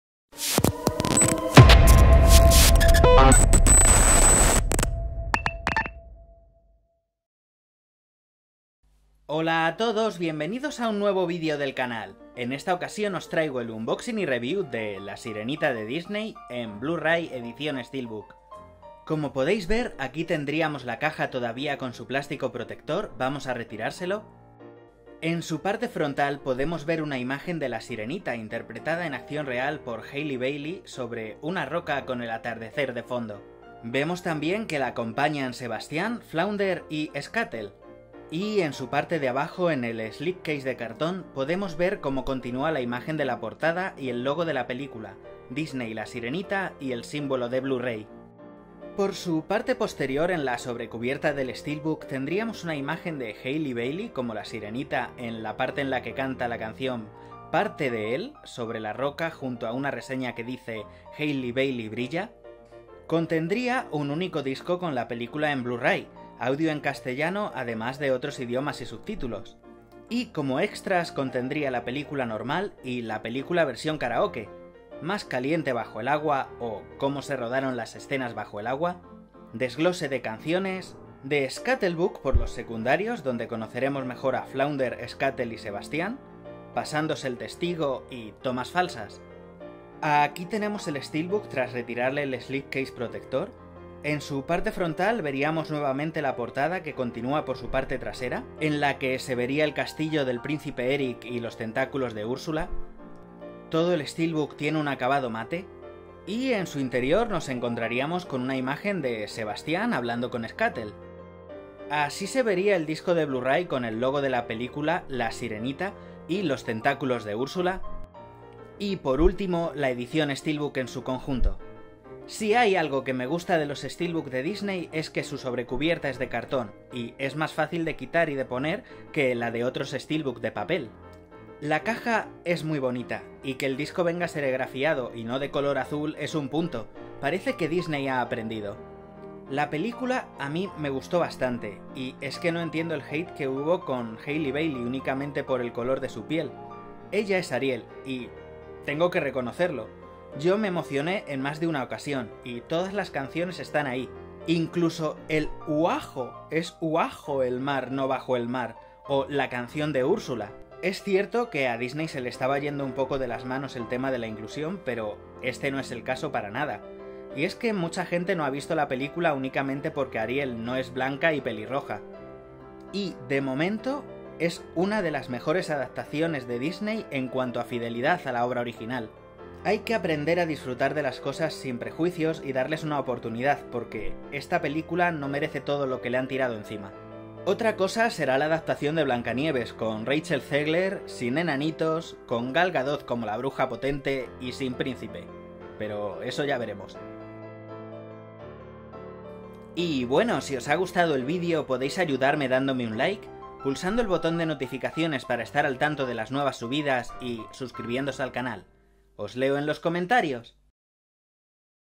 LA SIRENITA 2023 STEELBOOK UNBOXING&REVIEW
Unboxing de LA SIRENITA 2023 edición STEELBOOK que salió a la venta el 19/09/2023. Espero que os guste ;)

Podéis echarme una mano subtitulando los videos para que lleguen a mas gente desde este enlace:

DESCARGO DE RESPONSABILIDAD: Esta revisión representa mis opiniones sobre el producto comprado con mis propios fondos. Esta es una reseña, no un video promocional.